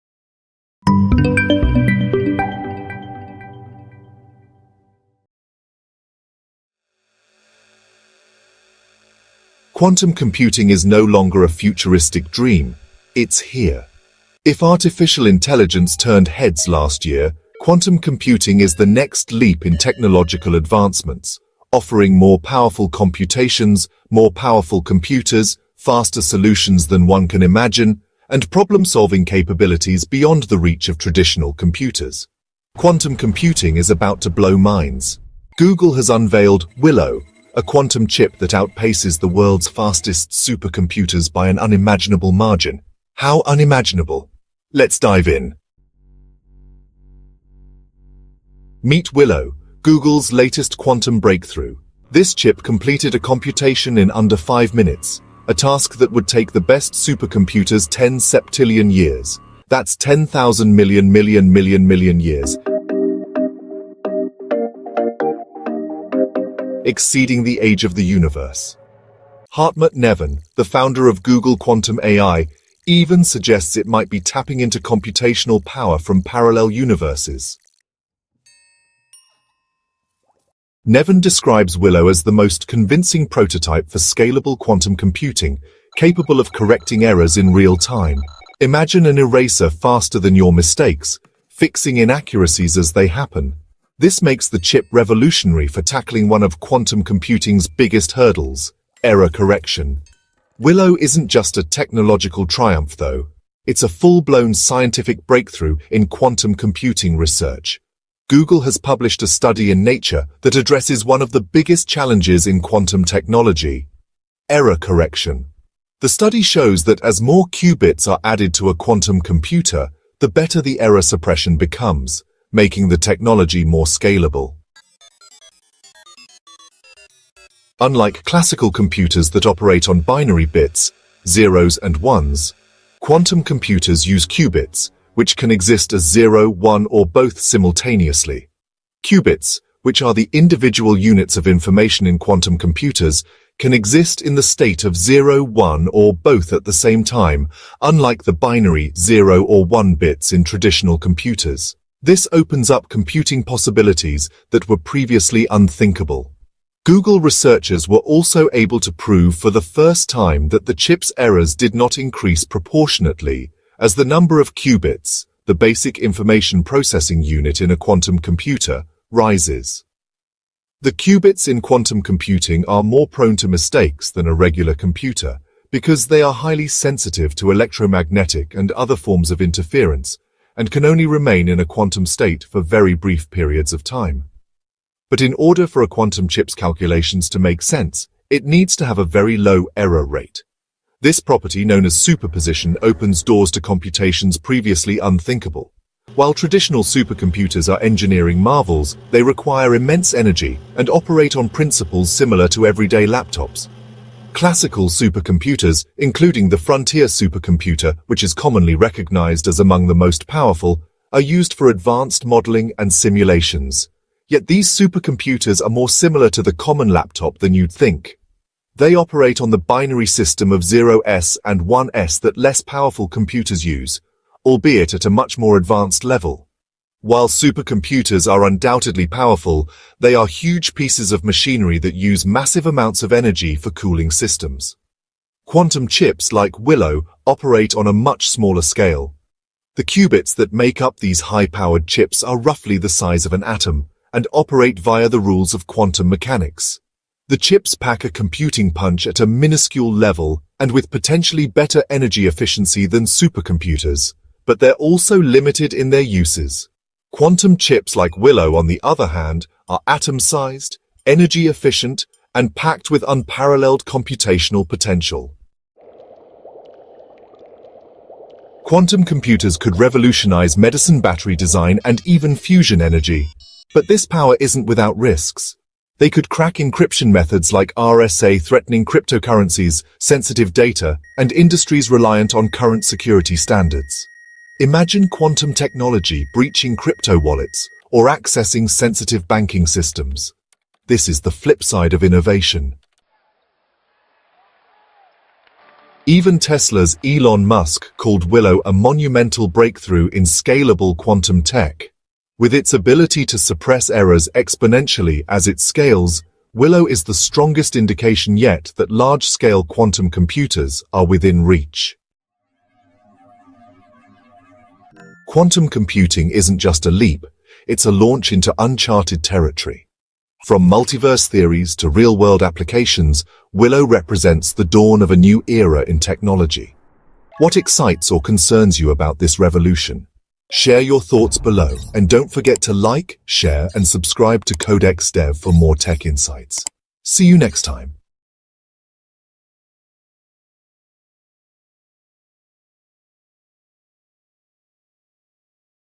Google’s Willow Quantum Chip – A Multiverse Breakthrough
Explore the groundbreaking advancements of quantum computing with Google's Willow Quantum Chip in our latest video! 🌌 In just under five minutes, Willow achieves computations that would take supercomputers 10 septillion years—exceeding the age of the universe! Discover how this revolutionary chip, touted by Hartmut Neven, is tackling error correction in real-time, unlocking new possibilities in fields like medicine and energy.

Join us on this journey through the multiverse of technology, where qubits revolutionize computation and challenge traditional security measures.

See Less

OUTLINE:
00:00:00 Introduction and Explanation of Willow Quantum Chip
00:00:39 Google's Quantum Breakthrough
00:01:07 Neven's Blog Post on Willow
00:01:54 Supercomputers vs Quantum Chips
00:04:14 Applications and Risks of Quantum Computing
00:04:42 Elon Musk's Reaction
00:05:01 Conclusion and Wrap-Up